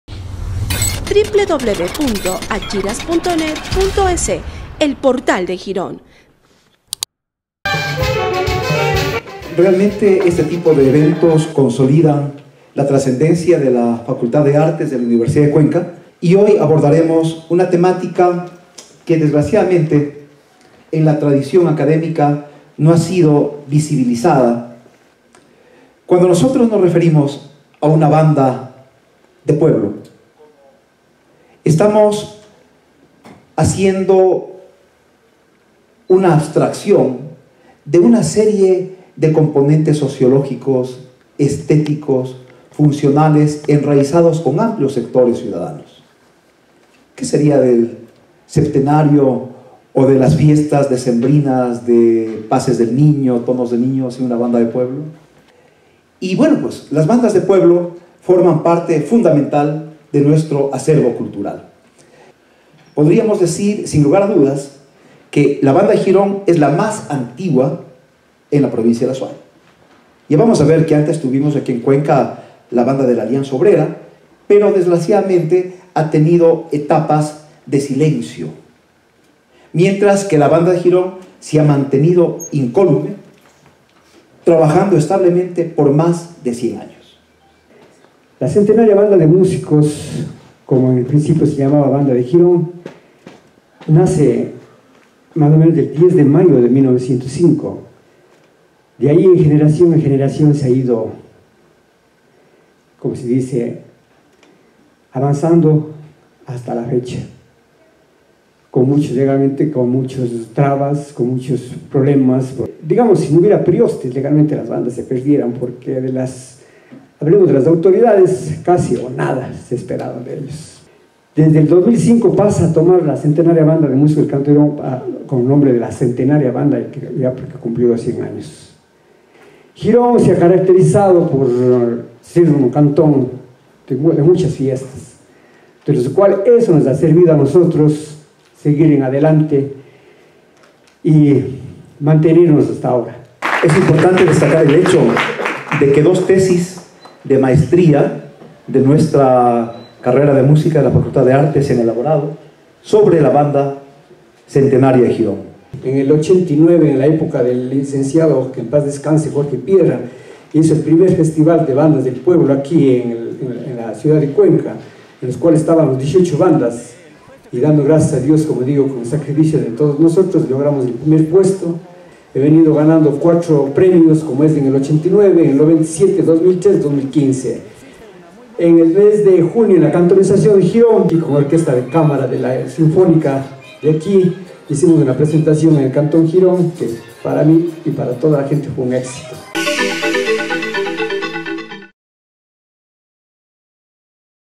En foro universitario, “La Centenaria Banda de Girón” es considerada la más antigua del Azuay
“Las bandas de pueblo forman parte fundamental de nuestro acervo cultural, podríamos decir sin lugar a dudas que, la banda de Girón es la más antigua, en la provincia del Azuay” así lo expresó el catedrático Universitario Carlos Freire durante un conversatorio en la Facultad de Artes de la Universidad de Cuenca, desarrollado con motivo del Festival de Artes Musicales. El director de “La Centenaria Banda de Músicos de Girón” Joaquín Pauta fue uno de los invitados a la mesa directiva.